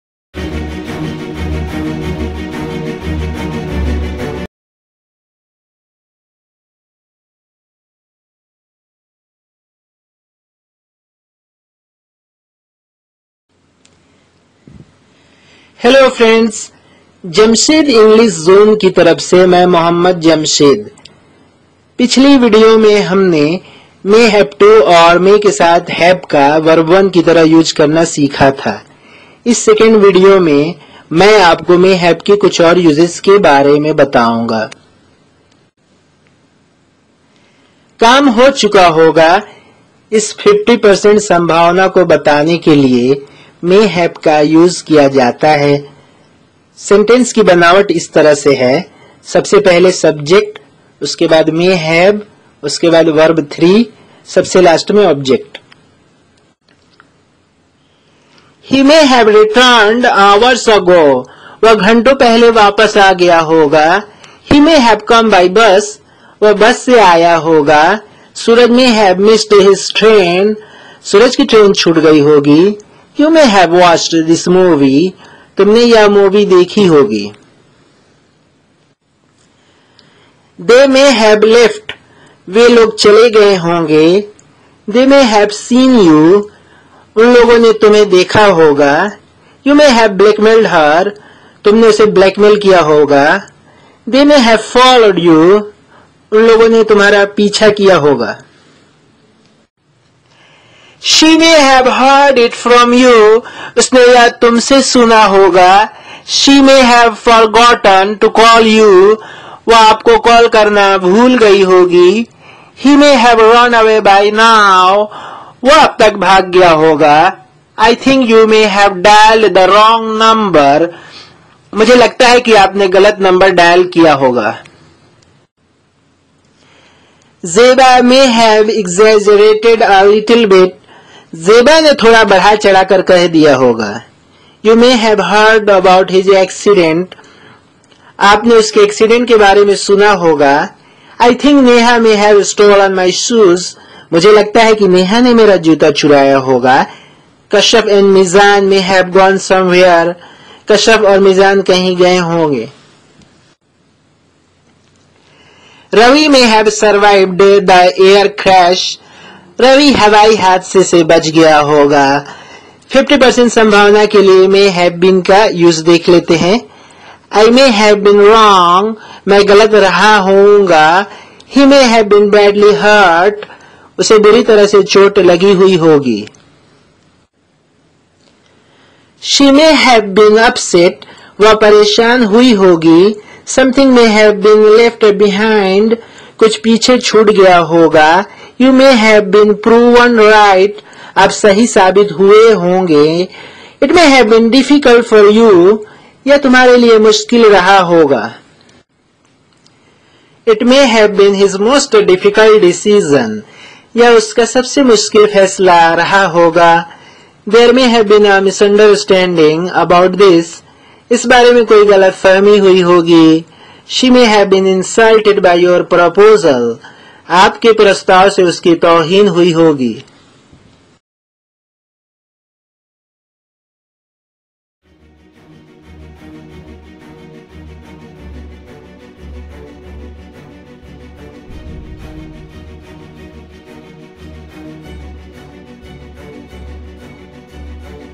Use of May have in Hindi
Use of may have in Hindi by Mohammad Jamshed.
Learn the use of May Have in Hindi /  Urdu (audio).
काम हो चुका होगा इस 50% Possibility-संभावना को बताने के लिए may have का use किया जाता है.
Subject + may have + V3 + Object.